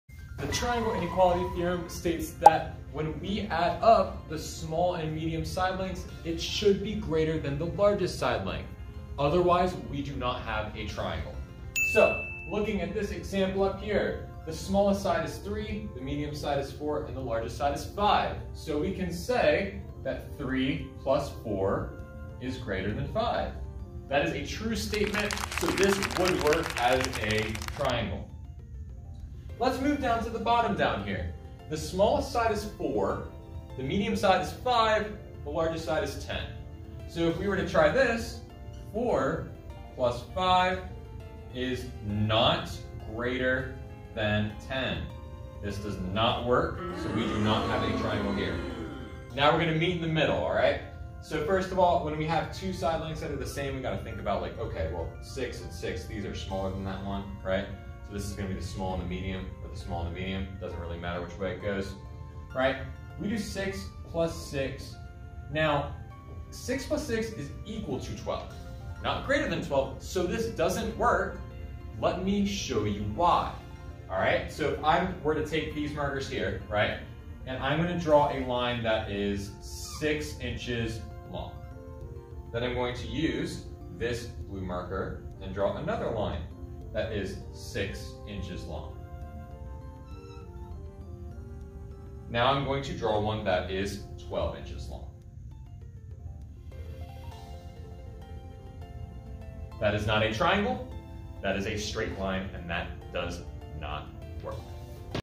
TRIANGLE INEQUALITY THEOREM made EASY
Tired of confusing math rules? 🤯 Master the Triangle Inequality Theorem in less than 2 minutes. You'll learn the one simple trick to know if three side lengths can actually form a triangle!